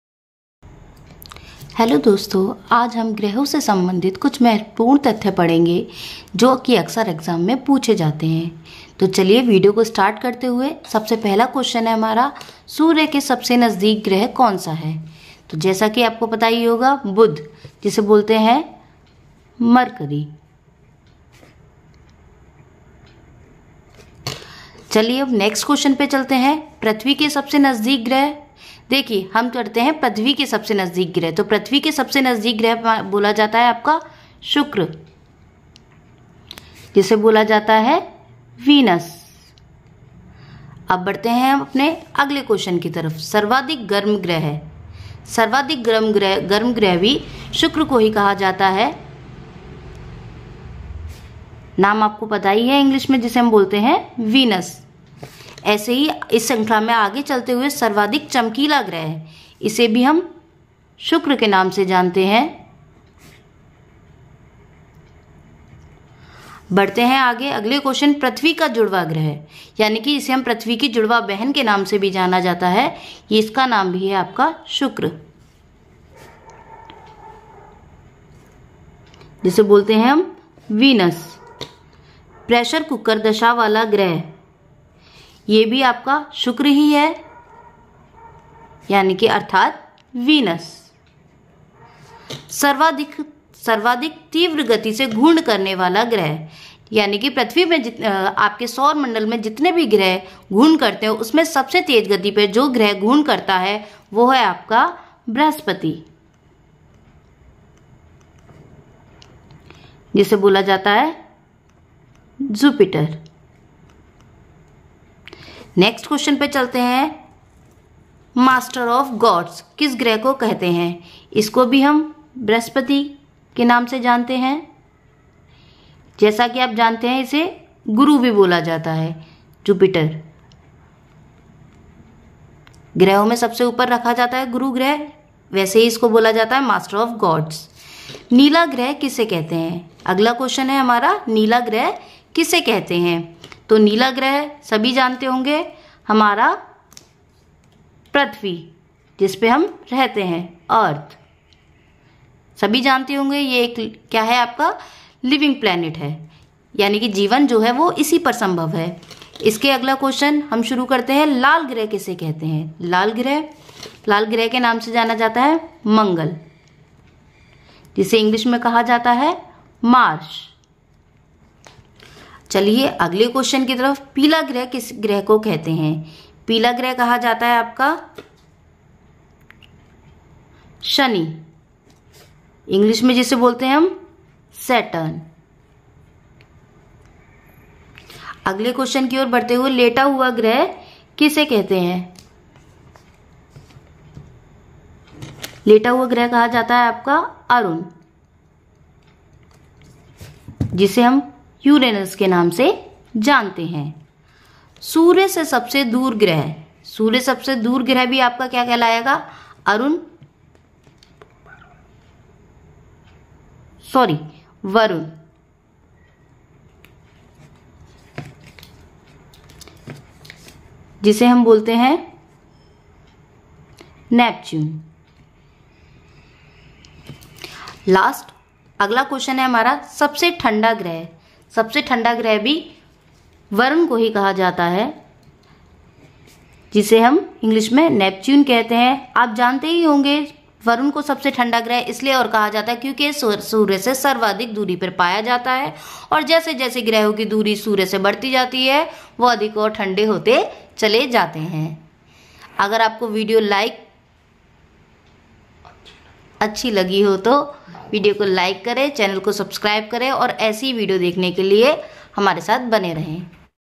ग्रहों से संबंधित प्रश्न || Solar System || भूगोल || Geography || Solar System Questions and Answers
सबसे अधिक गर्म ग्रह
लाल ग्रह कौन सा है
नीला ग्रह किस ग्रह को कहते हैं
पीला ग्रह किस ग्रह को कहते हैं
सूर्य के सबसे पास कौन सा ग्रह है
अधिक ठंडा ग्रह
पृथ्वी की बहन किस ग्रह को कहते हैं
red planet
yellow planet
blue planet
sister of earth
Hot planet
cold planet